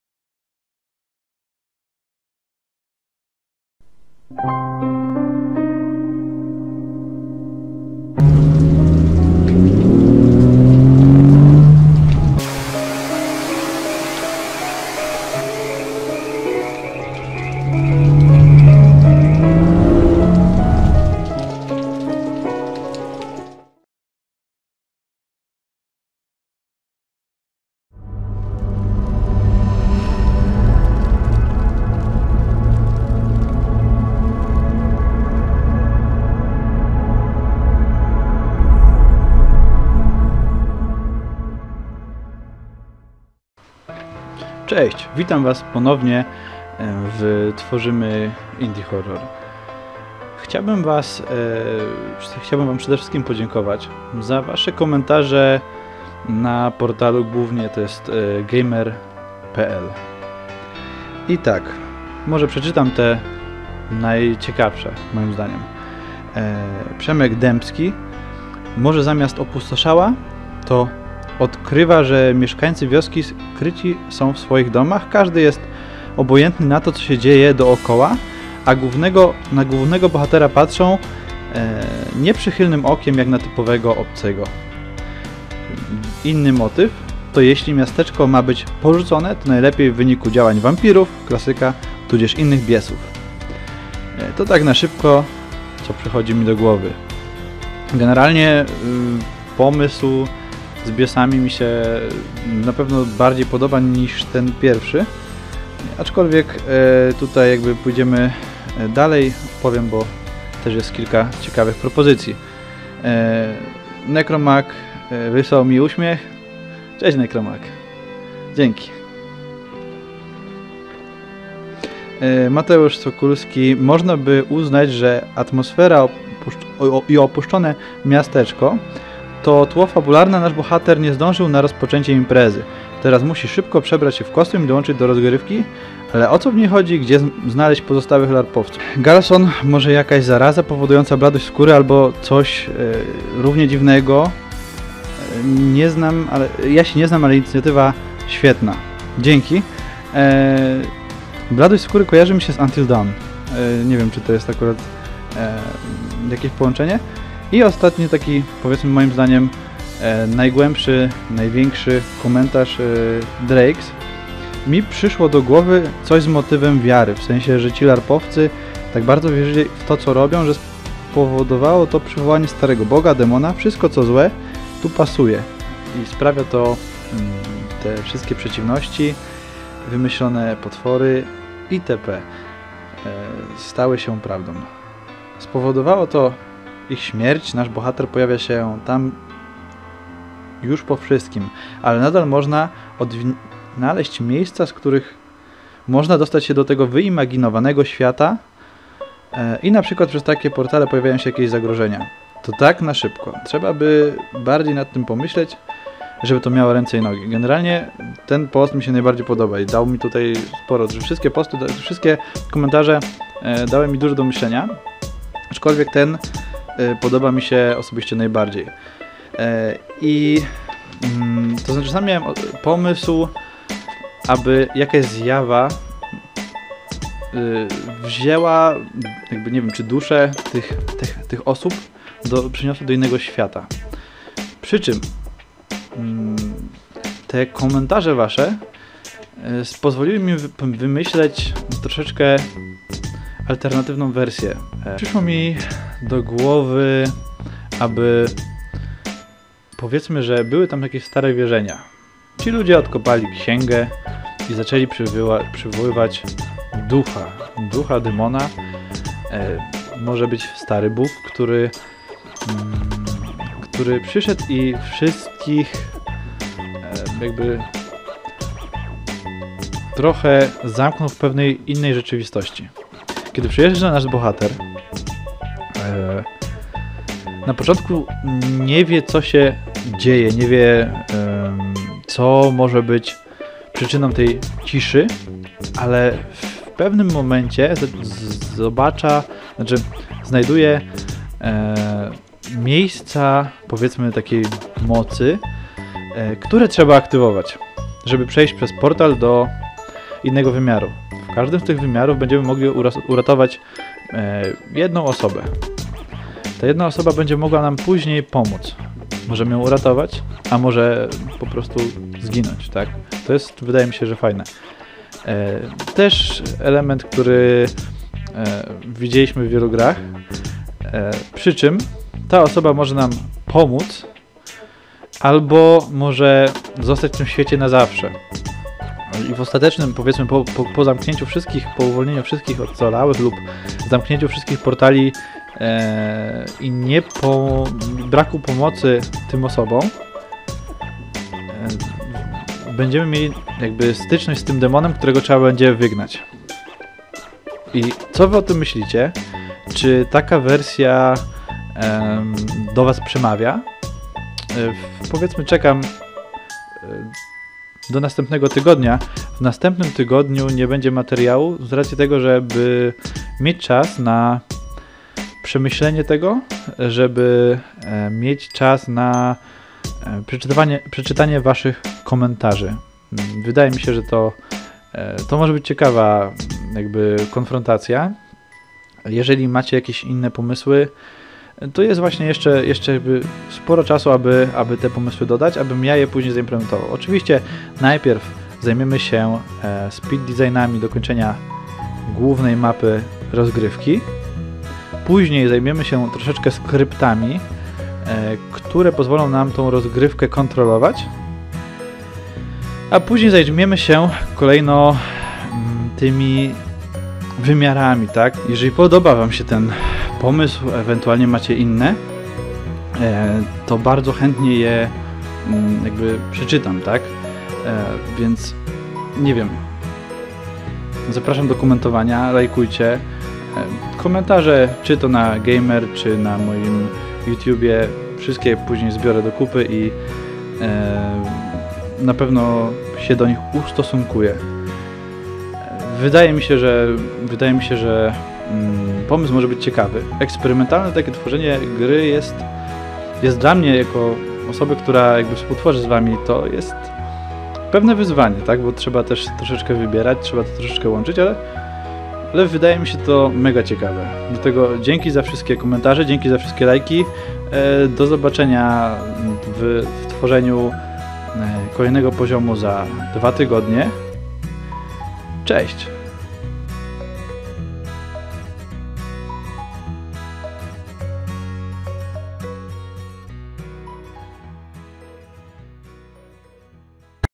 Back to medieval 4 - Q&A
Dziś chciałbym Wam zaprezentować trochę inny odcinek z serii – tworzymy grę… Dzisiaj odpowiem na wasze komentarze, oraz pomyślę co moża było by usprawnić. Czekam na komentarze! Lajk jest docenieniem mojej pracy, za każdy dziękuję.

Początek wspólnej przygody:
Nasz bohater to osoba zainteresowana światem średniowiecza i fantasy. Wybiera się na wydarzenie(LARP), gdzie będzie odgrywać rolę mieszkańca średniowiecznej wioski, lecz spóźnia się na inaugurację. Gdy pojawia się na miejscu mocno spóźniony to okazuje się, że osada jest całkowicie opustoszała. Nasz bohater czuje niepokój, ale nie podejrzewa jeszcze z jakimi zmorami się spotka tego wieczoru.

Na czym polega inicjatywa?

Tworzę indie horror – generalnie sprawa jest prosta. Ja buduję mapę, a w efekcie końcowym grę, gdzie Wy macie wpływ na jej fabułę. Jeśli jesteście zainteresowani to serdecznie zapraszam do dalszej części, w której przedstawię krótką historię rozpoczynającą serię gry.

Czekam na Wasze komentarze, w których napiszecie swoje propozycje  dalszej fabuły, zdarzeń. Zachęcam do współtworzenia ekscytującej gry.
Serdecznie dziękuję za okazywanie waszego zainteresowania seriom.